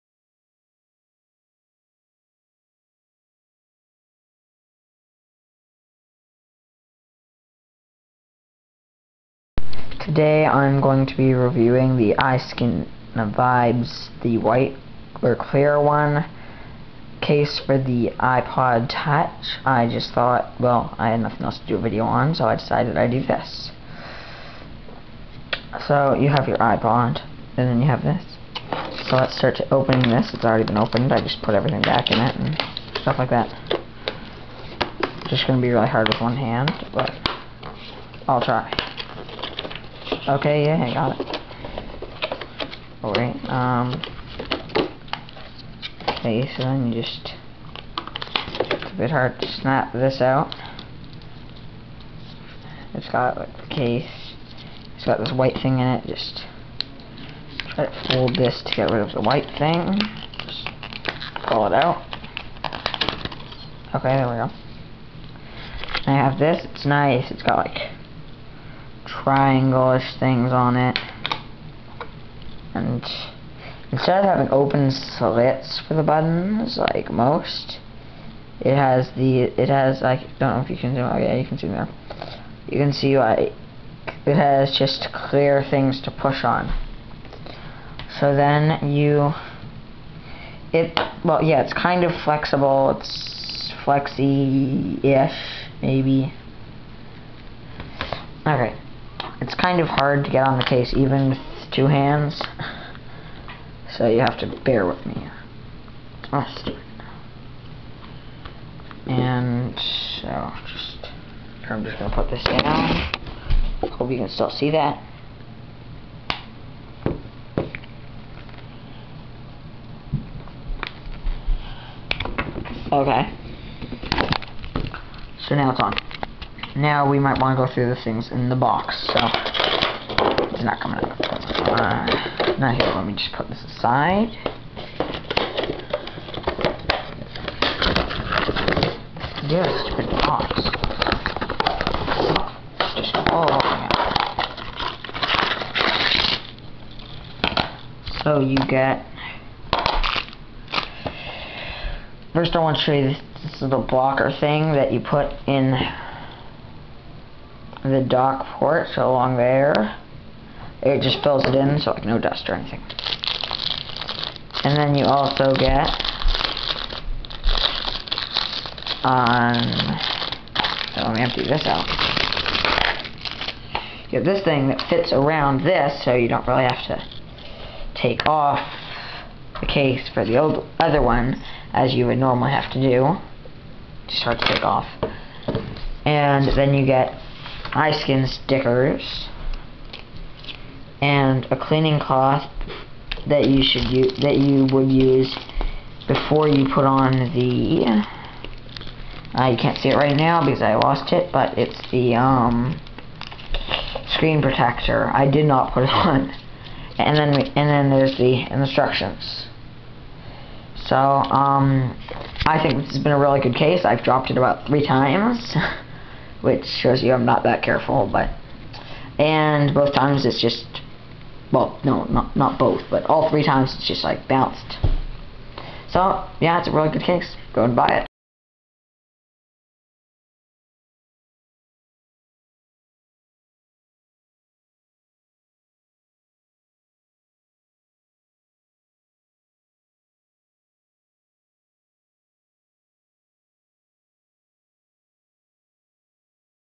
iskin vibes for ipod touch
A run though of the iskin vibes case for ipod touch